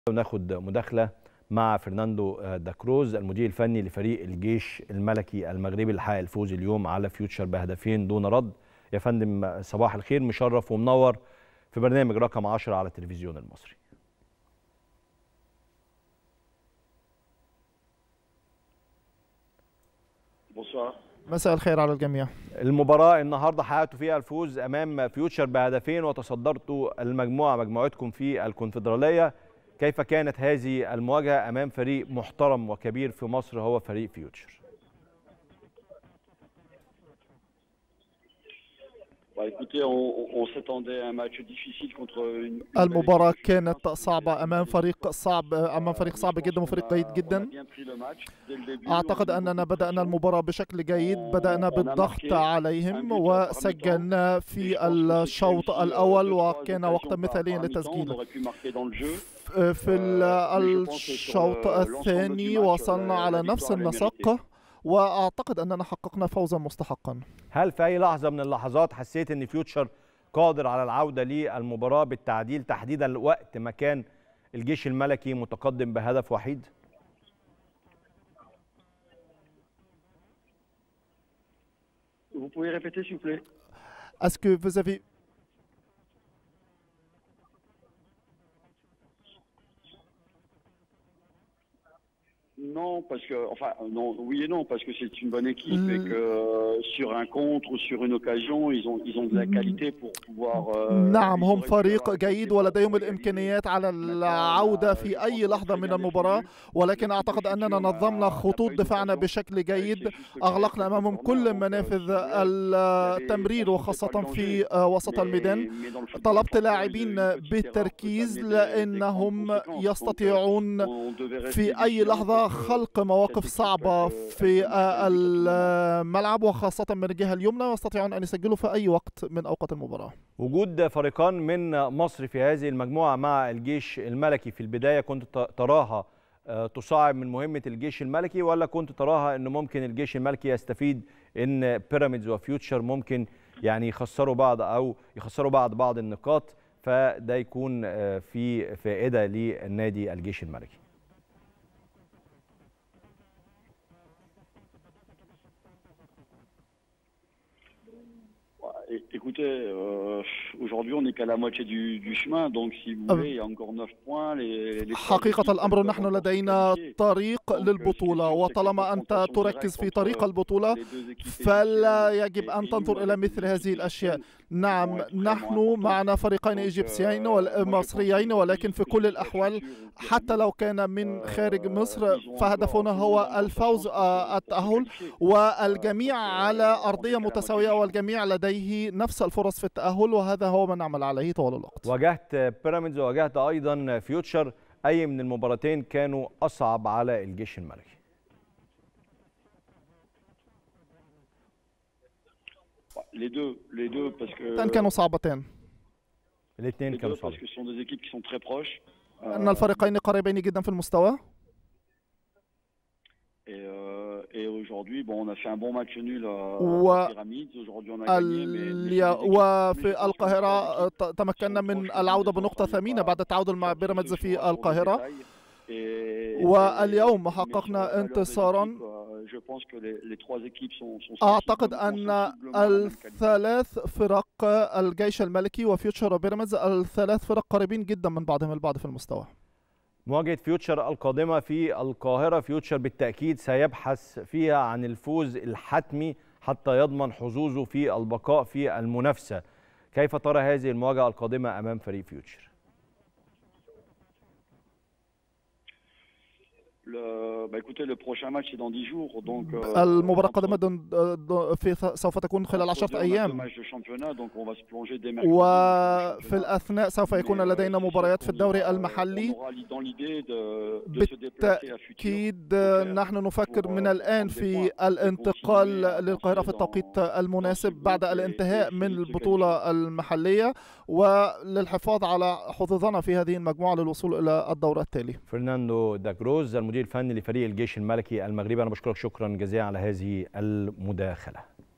فيرناندو دا كروز: مباراة فيوتشر كانت صعبة للغاية، واجهنا فريق جيد ومنظم للغاية
فيرناندو دا كروز: مباراة فيوتشر كانت صعبة للغاية، واجهنا فريق جيد ومنظم للغاية، بدأنا بالضغط عليهم جيدًا وسجلنا هدفًا في وقت جيد.